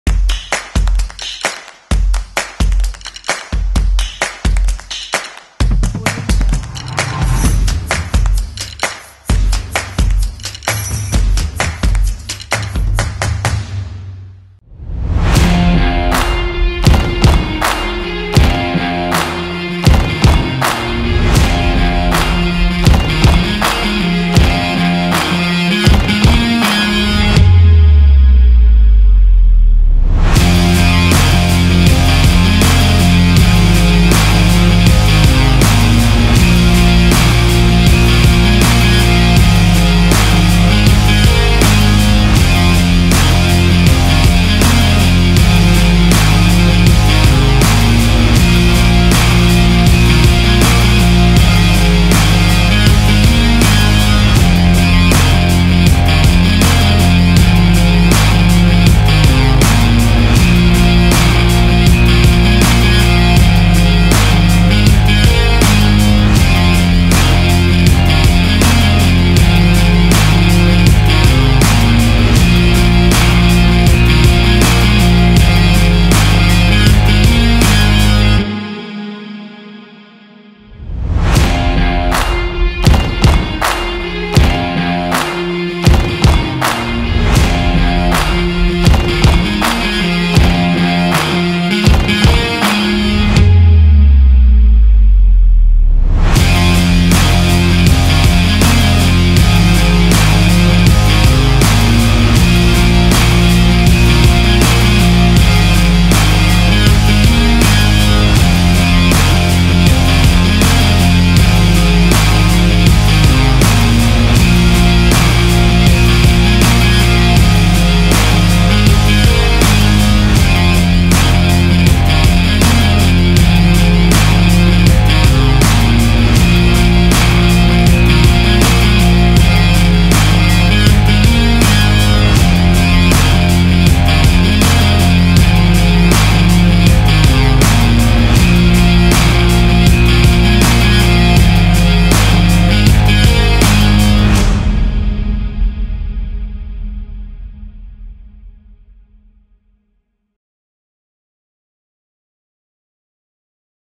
La Métaphore - Episode 11 - Blois vs Denain - Pro B - 26-01-2020
En partenariat avec VSF, retrouvez l'épisode 11 de la Métaphore qui revient sur le match qui a opposé l'ADA Blois Basket 41 à Denain lors de la 16ème journée du championnat de France de Basket de Pro B !

Une métaphore en hommage à Kobe Bryant, décédé tragiquement le jour de cette rencontre.